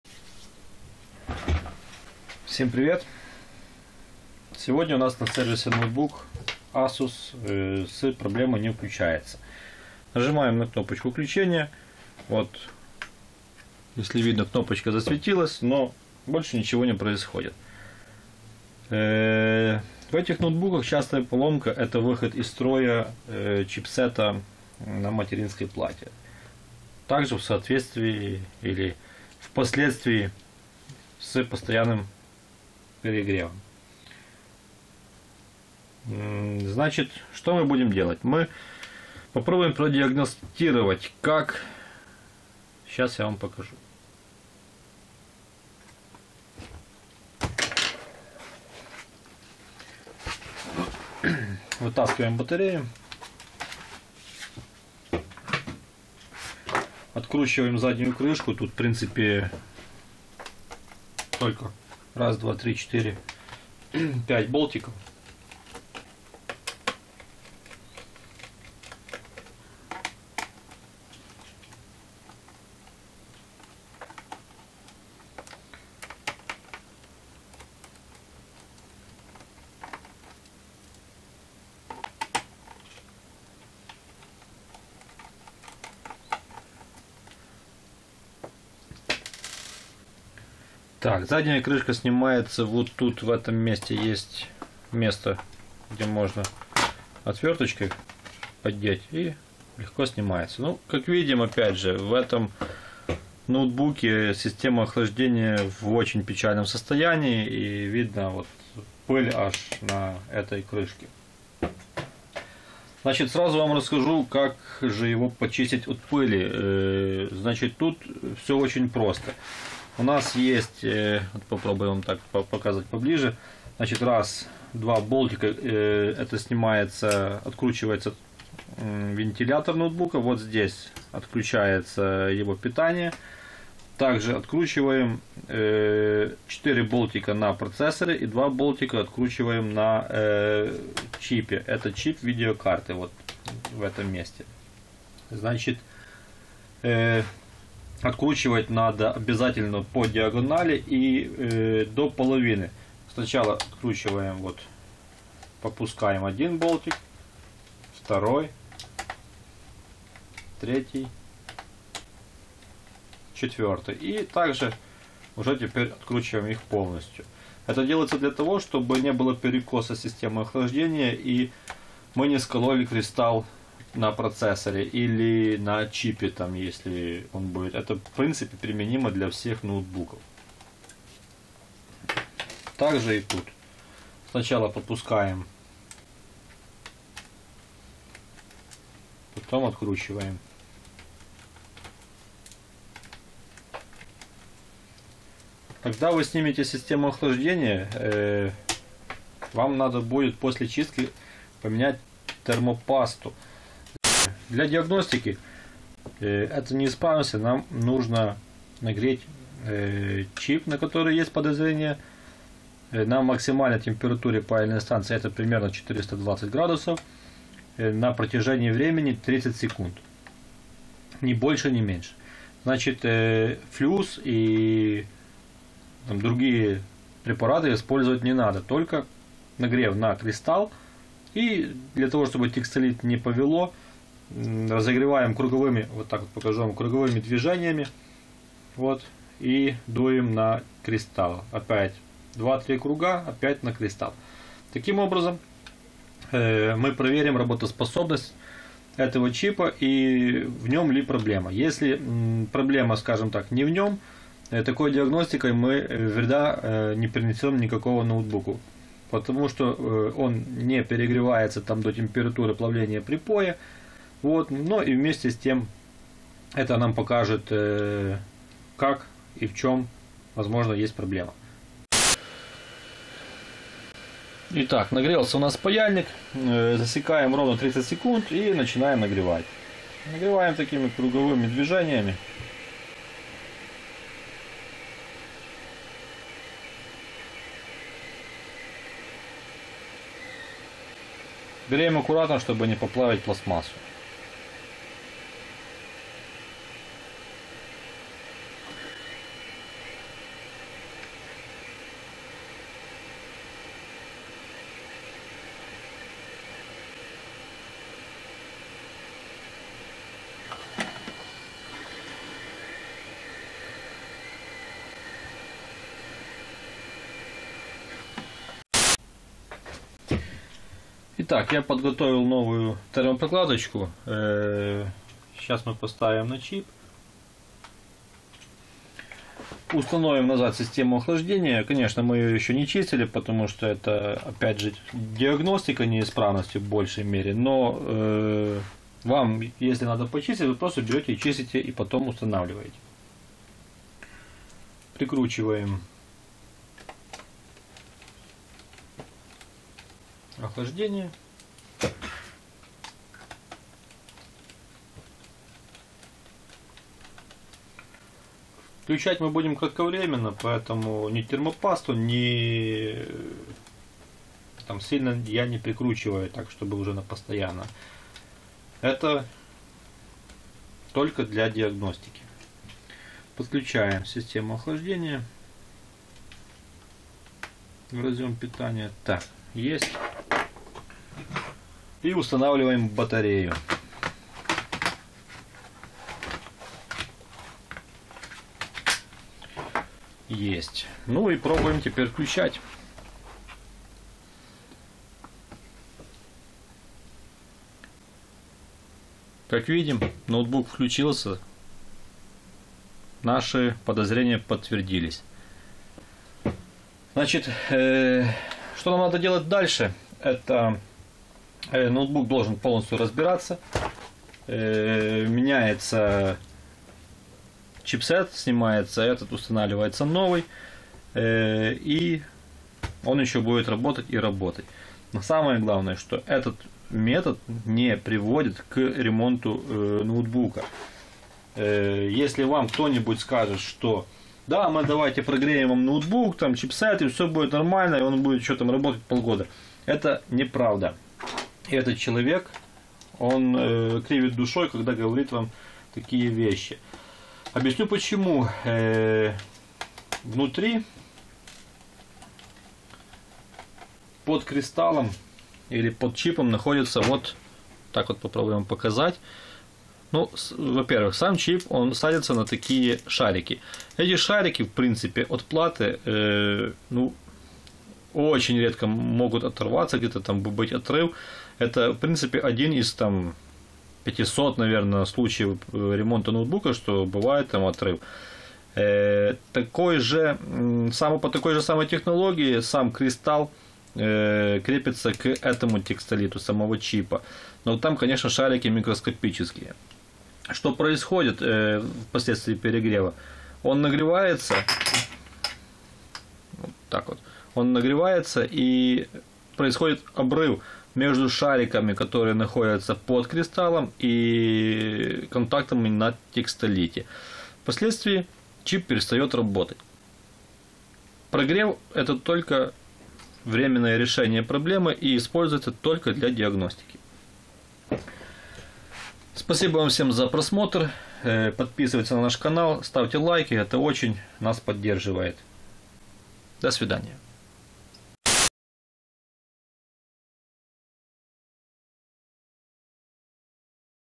✅ Чистка ноутбука Asus и диагностика неисправности
В этом видео я покажу вам как почистить систему охлаждения в ноутбуке Asus. Также маленький ликбез по чипах, причинах выхода их из строя и диагностика выхода чипа из строя.

Ноутбук Майстер - Ремонт ноутбуків та планшетів  в Дрогобичі - nbmaster.com.ua